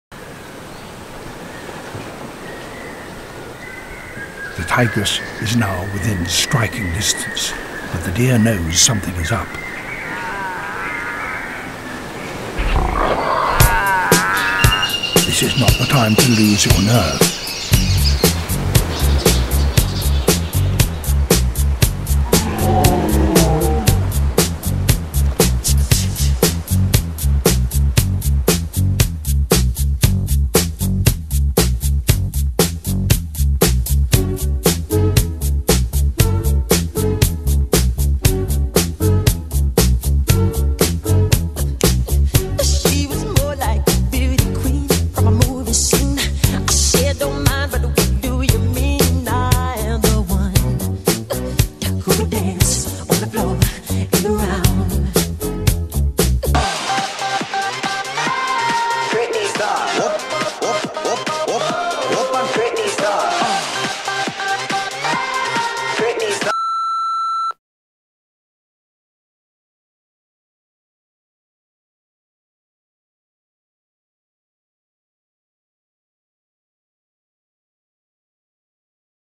Tiger Moonwalking through Blackpool (TigerMoonwalk)
A tiger has been spotted moonwalking around Blackpool's attractions.
Let us know if you spot him.